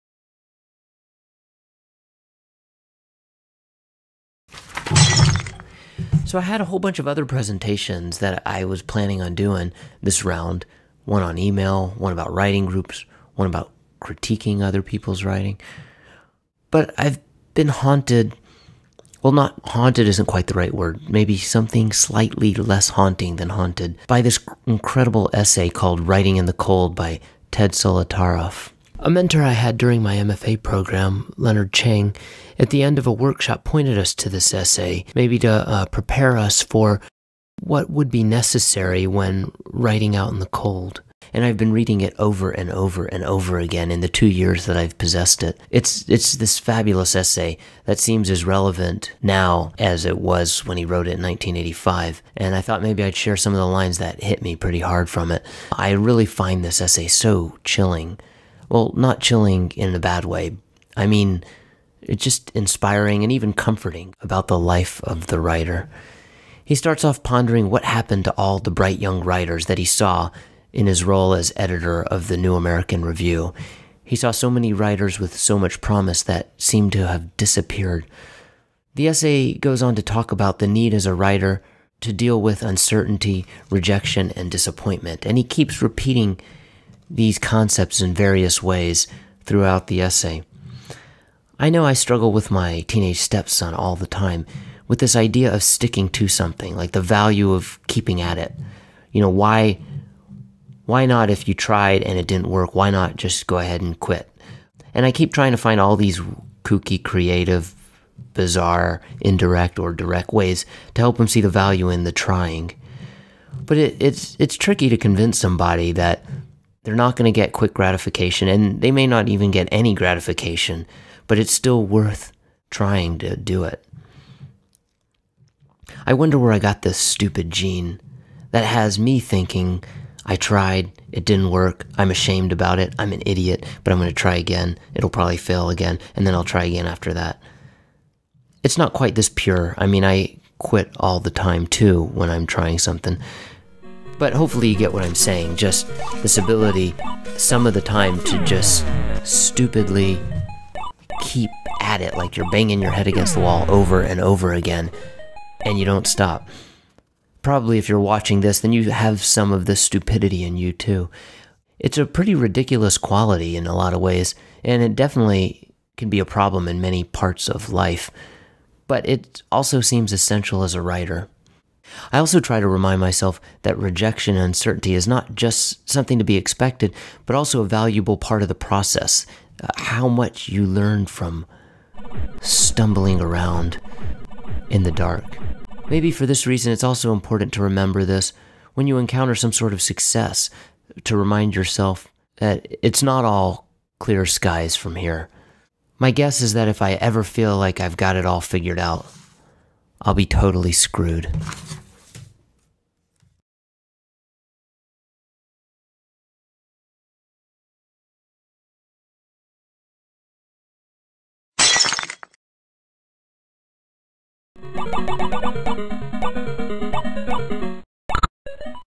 Writing in the Cold (Failed Writer #5)
Here is my video tribute to an incredible essay about the essential qualities of a writer by Ted Solotaroff. I apologize in advance for the comparison of the writing life to a game of Frogger.